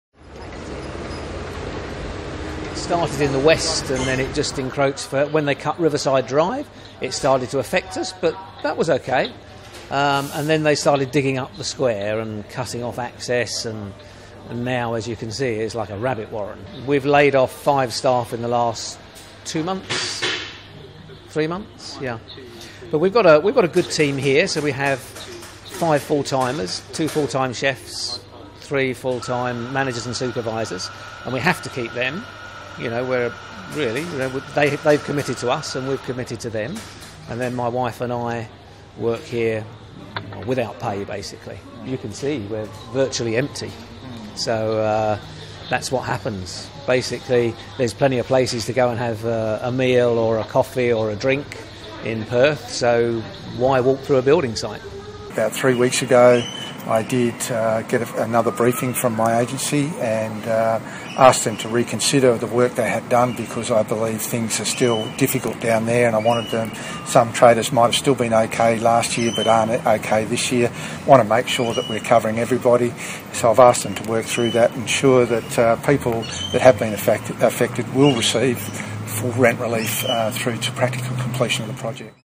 Struggle for Barrack Square business 01:22
Riverside cafe owner Bryan Molnar says his business is being impacted from construction works at Elizabeth Quay.

    Struggle for Barrack Square business
    01:22

    01:22

    01:22